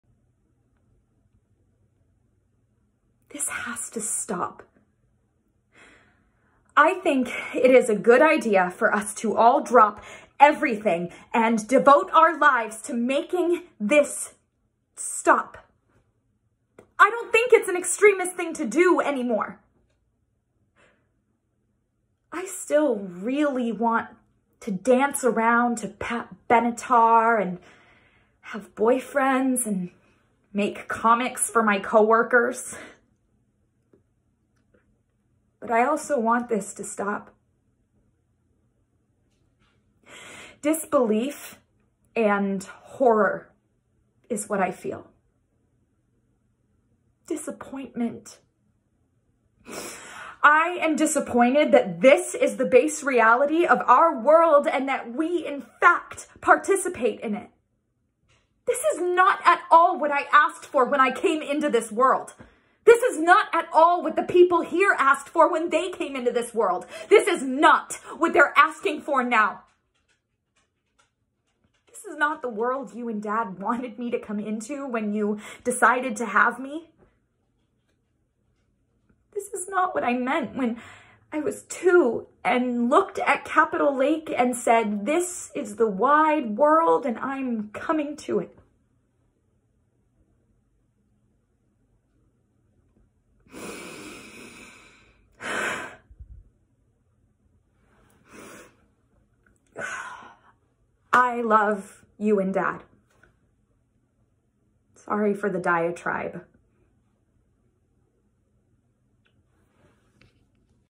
Rachel Corrie Monologue (My Name is Rachel Corrie)
My Name is Rachel Corrie—
written by Rachel Corrie
edited by Alan Rickman and Katharine Viner
Role: Rachel Corrie

Rachel Corrie was a human rights activist and writer from Olympia, WA who was heavily involved in fighting injustice during the Palestinian-Israeli conflict on the Gaza Strip in 2003.

If you want to do something to help relieve some of the insanity we are facing in the world, consider donating to the Lebanese Red Cross. They are working hard to take care of their people after the explosion in Beirut.

Originally posted on Instagram and Facebook, August 10th, 2020.